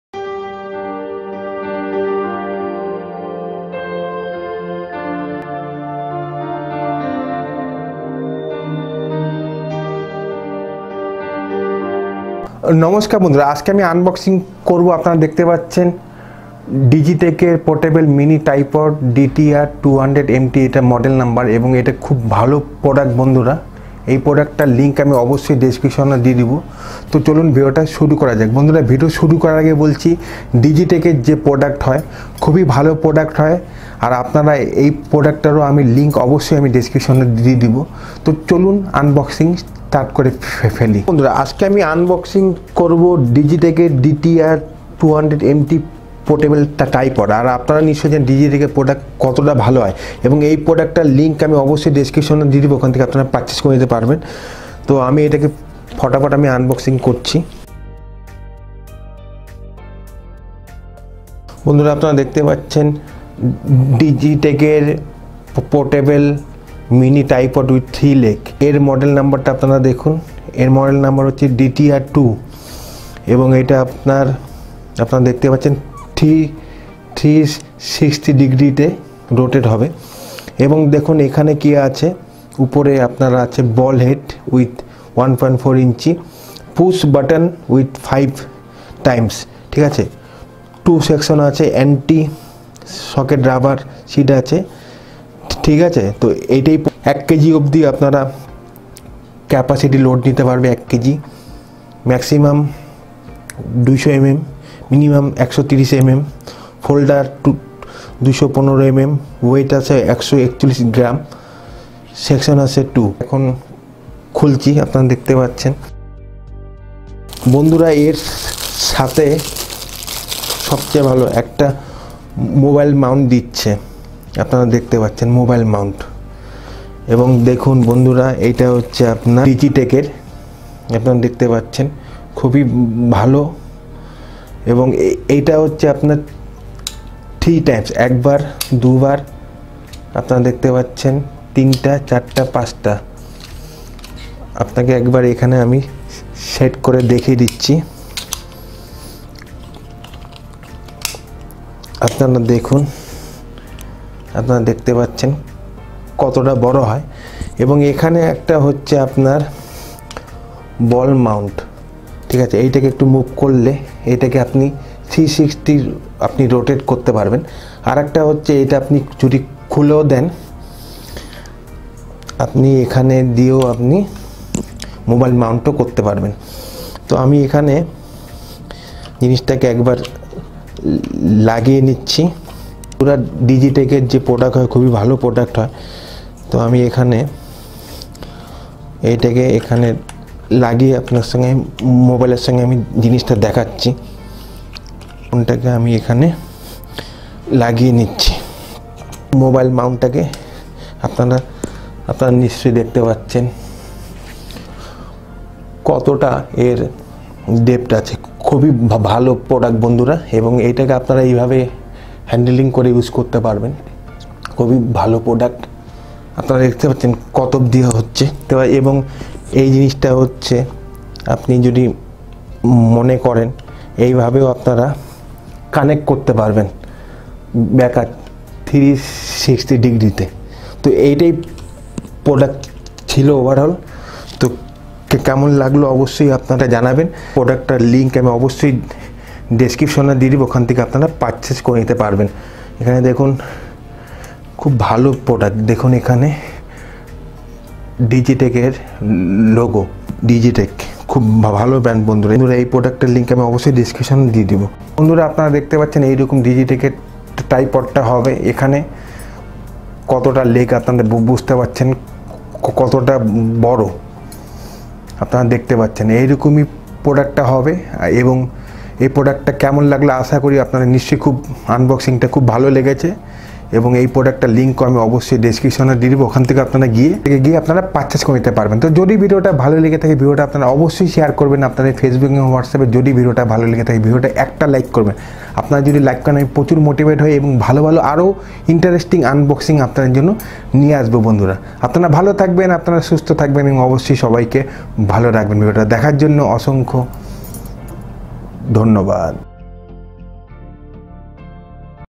digitek dtr 200 mt portable mini tripod with 3 leg base unboxing
Hello, Friend in this video i unboxing digitek dtr 200 mt portable mini tripod .